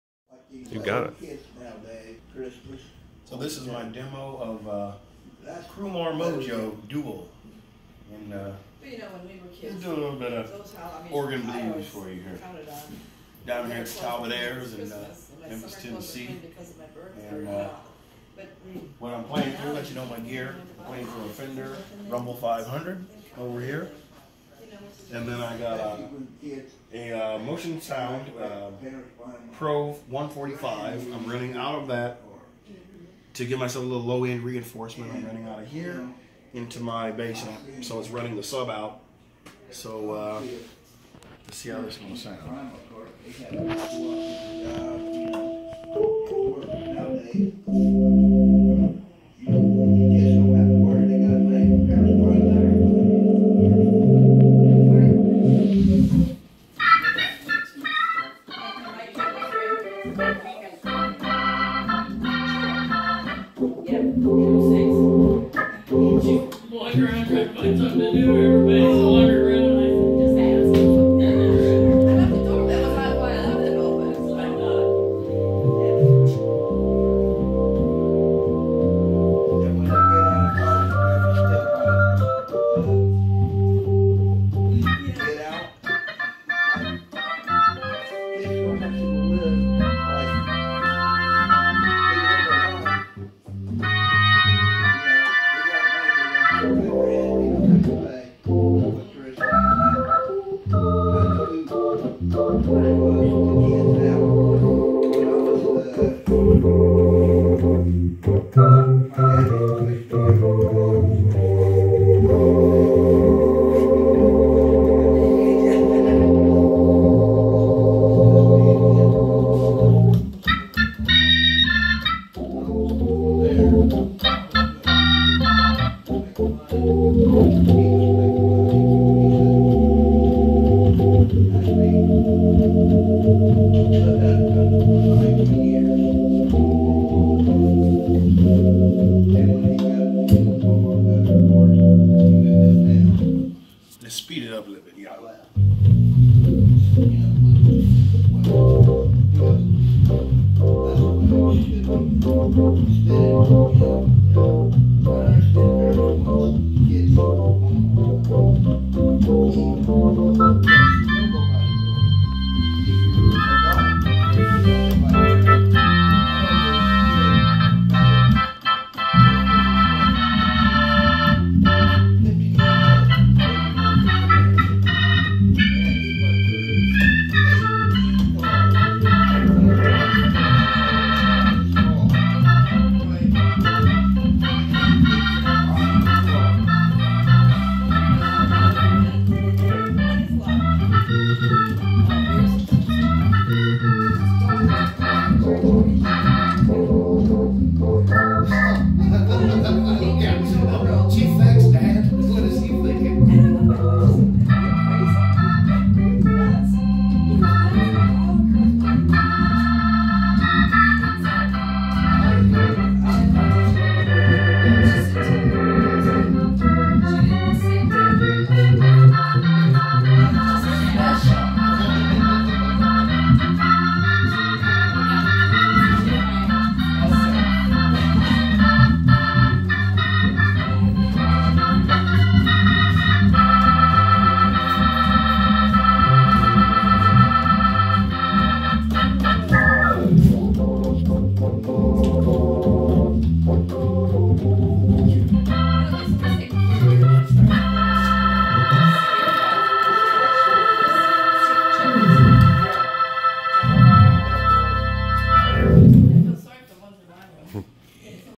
Crumar Mojo Dual/Fender Rumble 500/Motion Sound Pro 145
Just a short demo of my organ setup